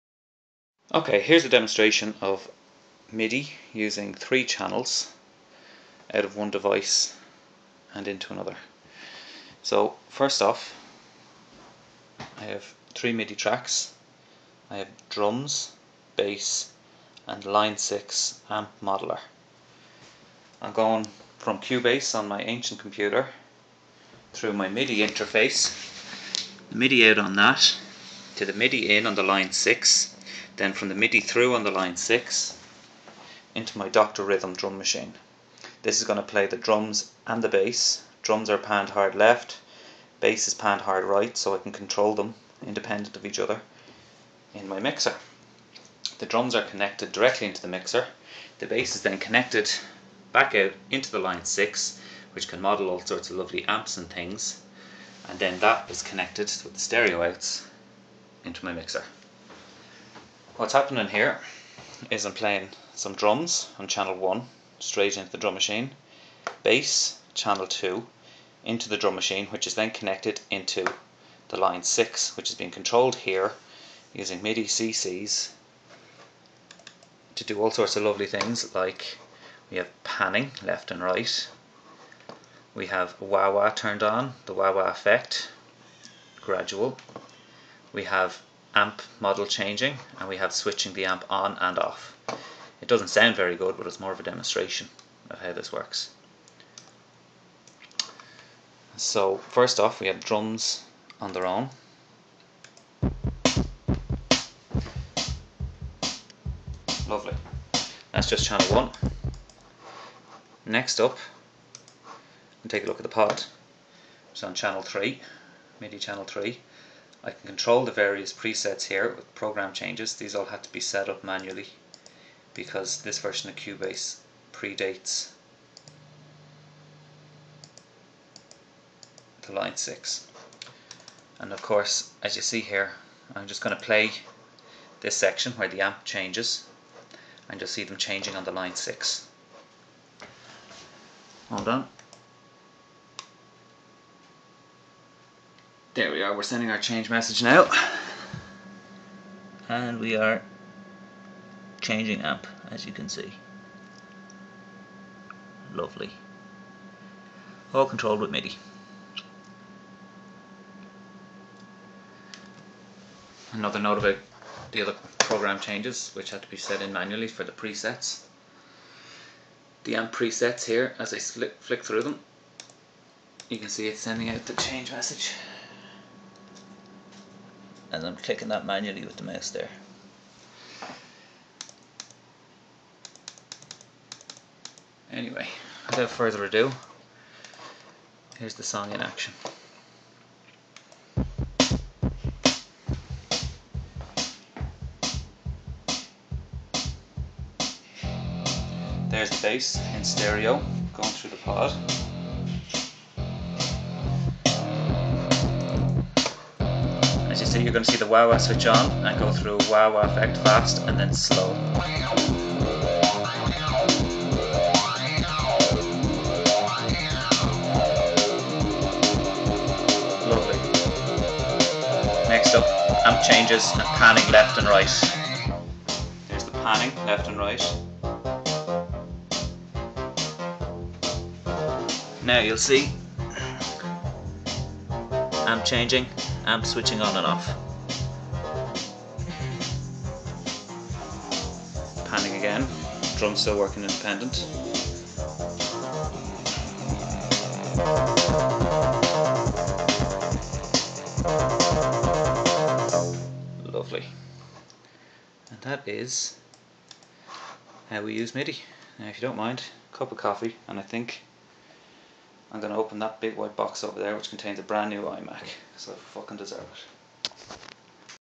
MIDI Demo
A quick demo of a MIDI setup from November. The DAW (running an ancient version of Cubase) is controlling the drum machine, which is also playing bass -one output for each instrument. The bass output of the drum machine is connected to the Line 6 Pod xt which
is also controlled by Cubase, this time using MIDI CC and program change data.